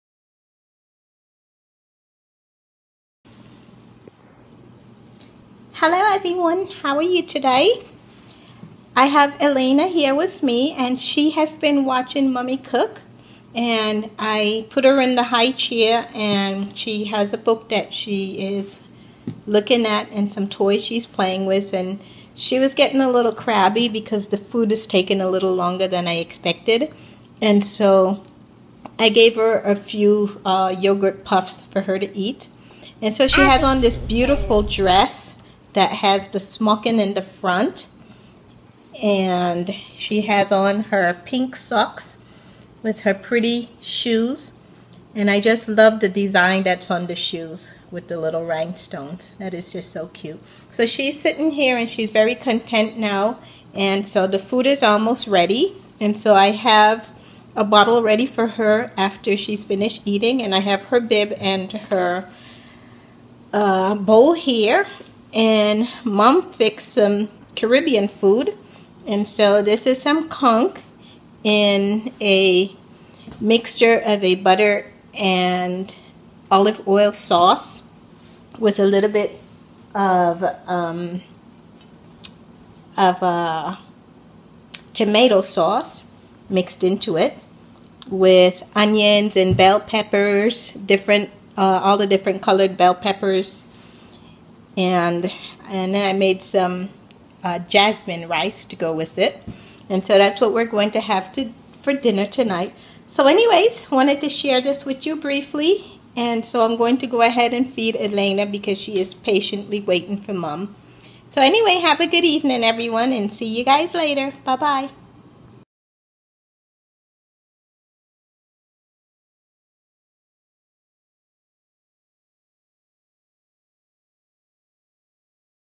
Reborn Toddler Elayna in highchair
Reborn toddler Elayna watching Mommy cook